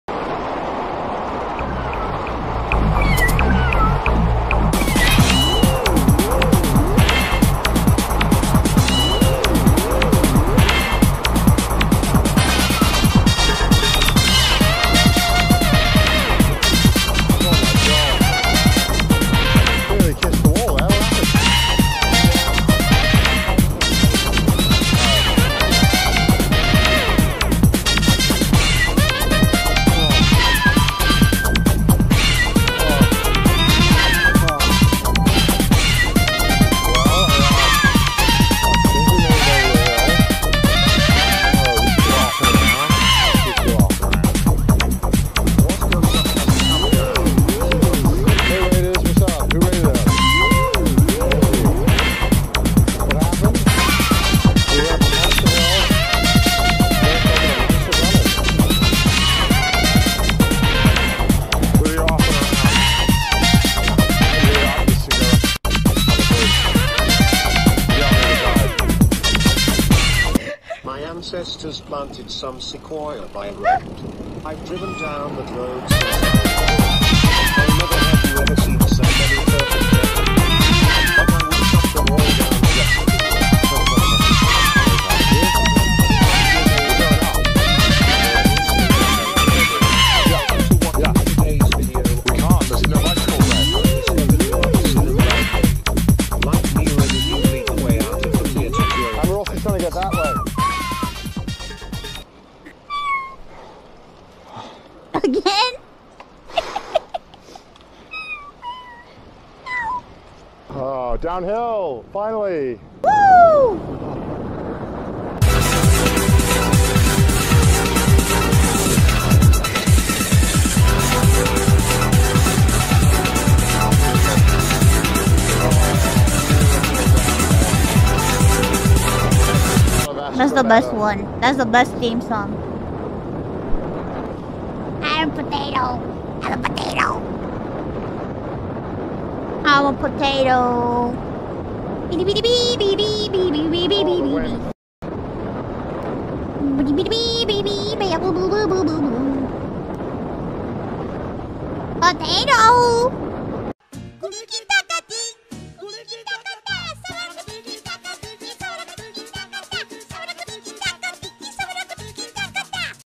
Ironmouse is a Potato
Ironmouse was dancing along to a cat meowing song then enjoyed a bit of a downhill with Connor before having a bit of a tangent about her being a Potato.